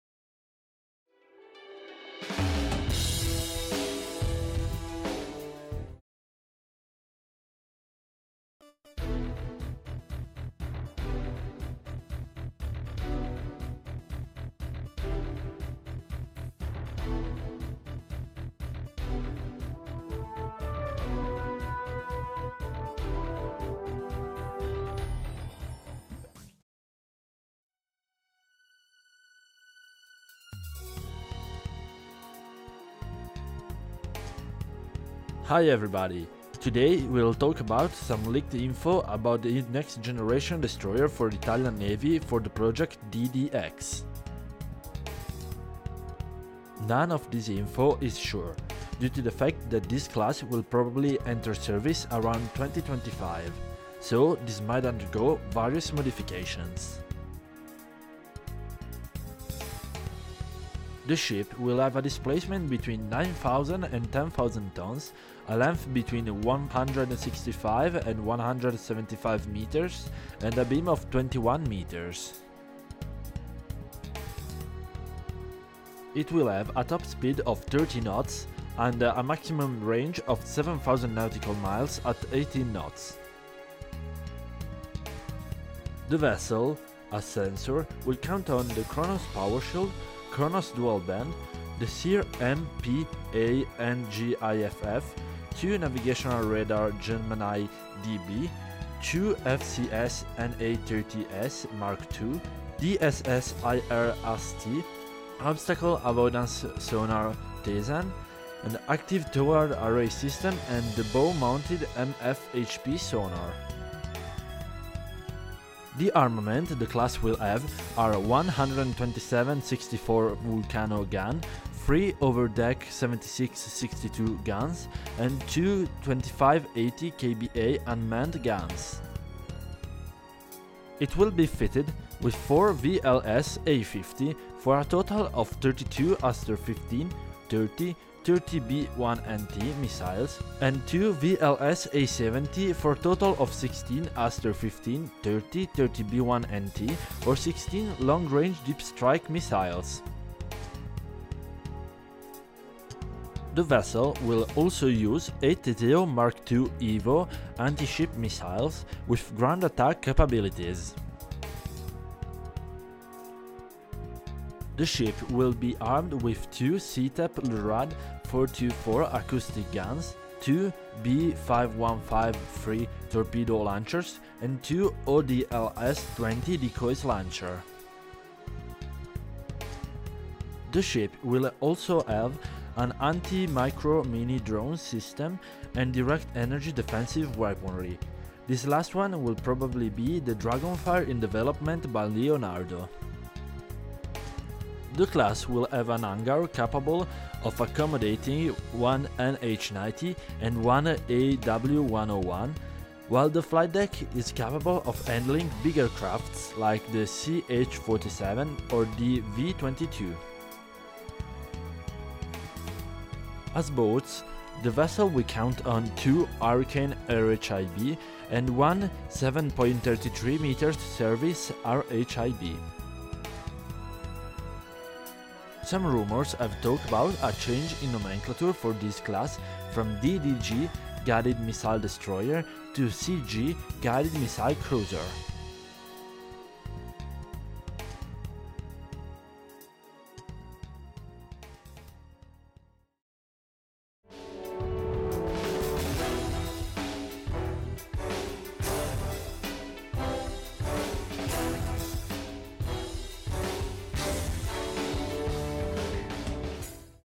Next Generation Destroyer - DDX Project News 11/2018
Hi everybody,
today we will talk about some leaked info about the next generation destroyer for the Italian Navy for the project DDX.
None of this info is sure, due to the fact that this class will probably enter service around 2025, so this might undergo various modifications.